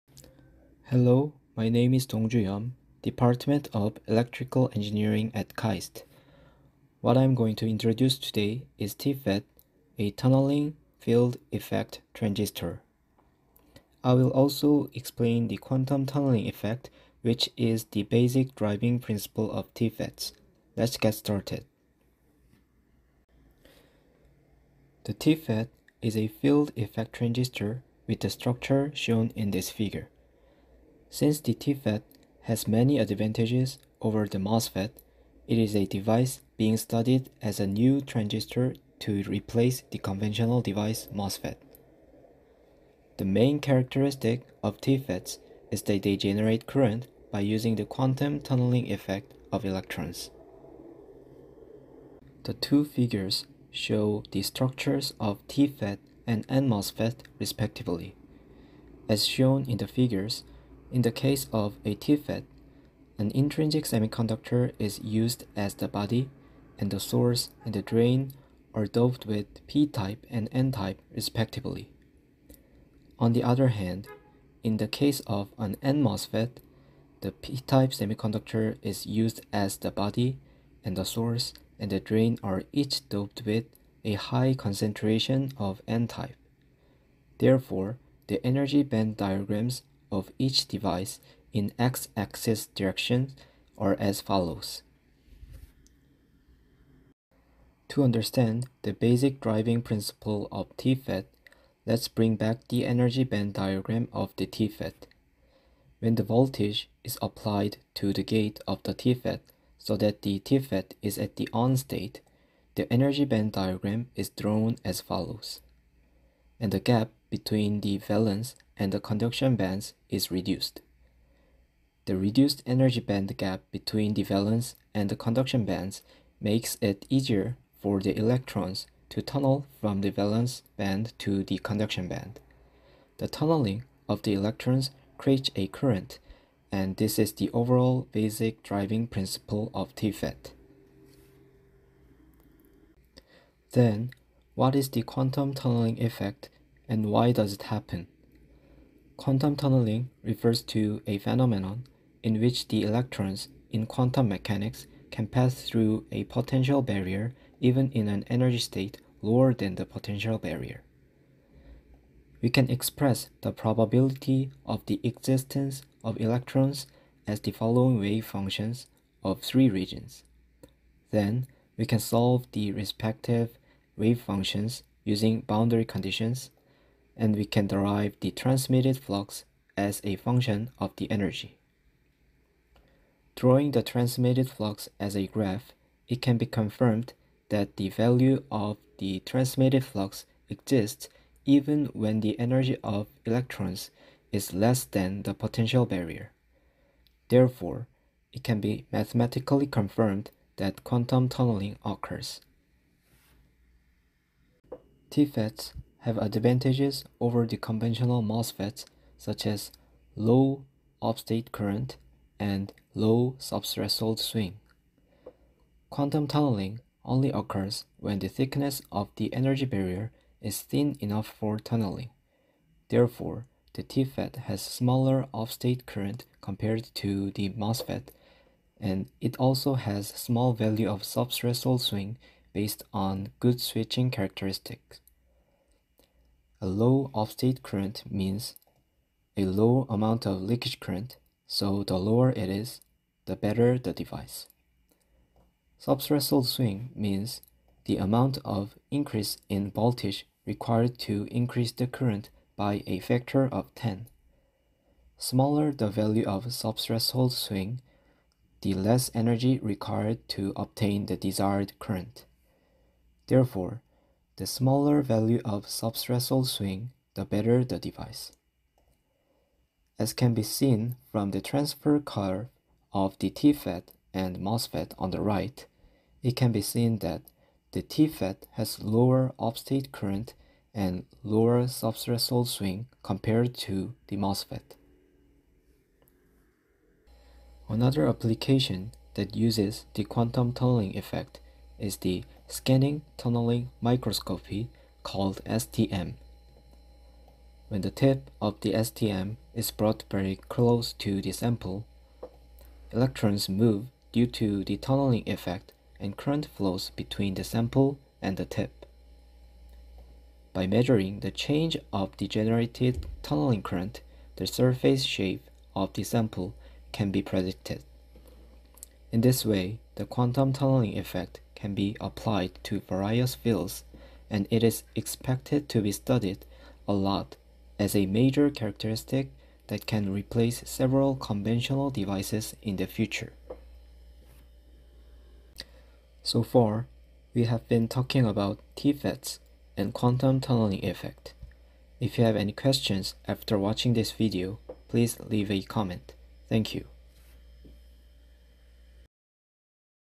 [Project] TFET(Tunneling Field Effect Transistor) and Quantum Tunneling Effect
This video provides an overview of TFETs and quantum tunneling effects.

KAIST MS310 Youtube Project
20180388 Dongju Yeom (염동주)